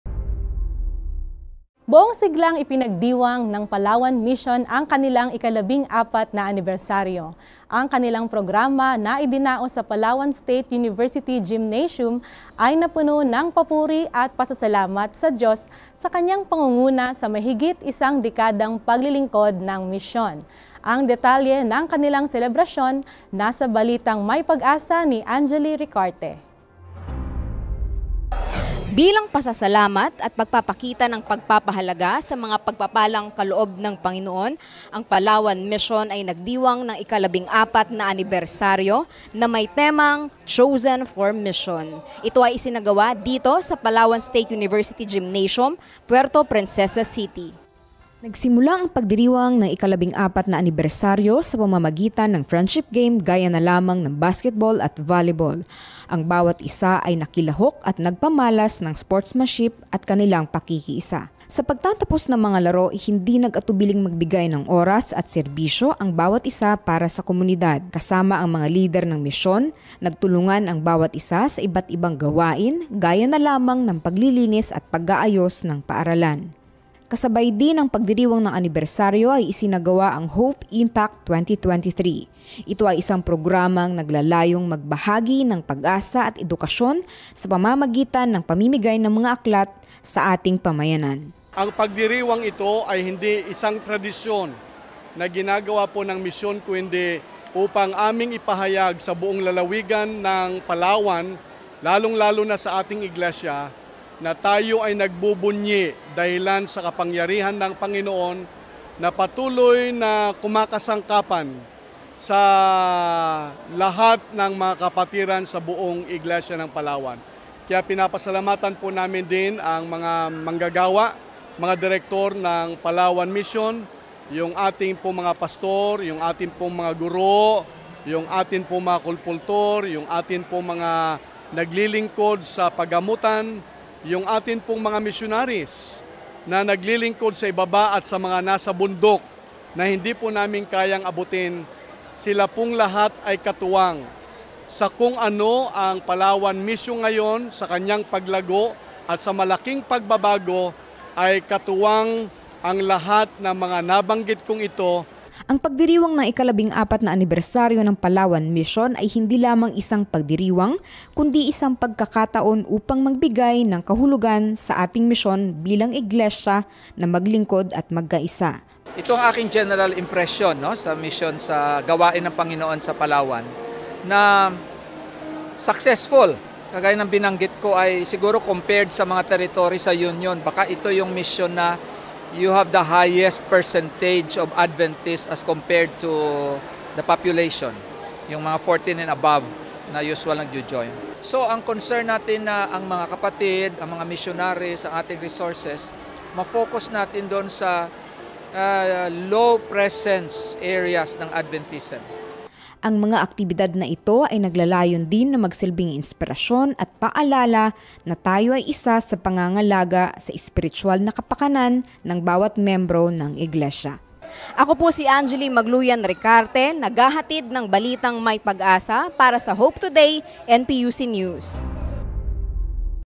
Palawan Mission masayang ipinagdiwang ang 14 taon na pangunguna, pagpapala ng Diyos
Buong siglang ipinagdiwang ng Palawan Mission ang kanilang ika-14 na anibersaryo.
Ang kanilang programa na idinaos sa Palawan State University Gymnasium ay napuno ng papuri at pasasalamat sa Diyos sa kanyang pangunguna sa mahigit isang dekadang paglilingkod ng misyon.
Ang detalye ng kanilang selebrasyon, nasa balitang may pag-asa ni Angelie Magluyan- Ricarte…